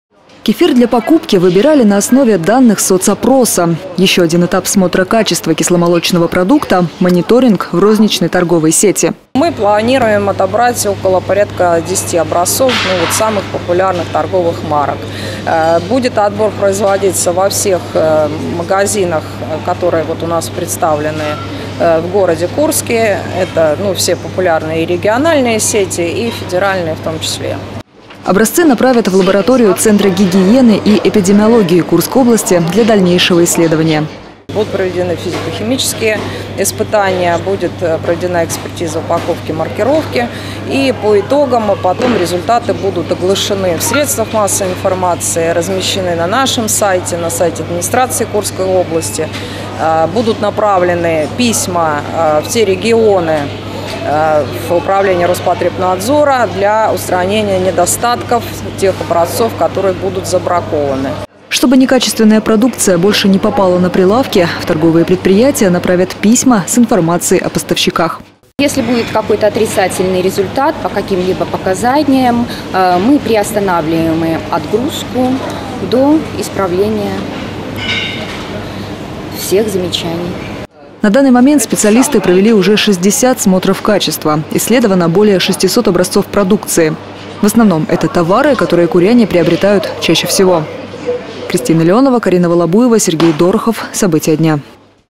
В Курске провели контрольную закупку кефира
В Курске прошла контрольная закупка кефира. Это первый этап сравнительного смотра качества. Его проводит комитет промышленности, торговли и предпринимательства региона совместно с центром контроля качества.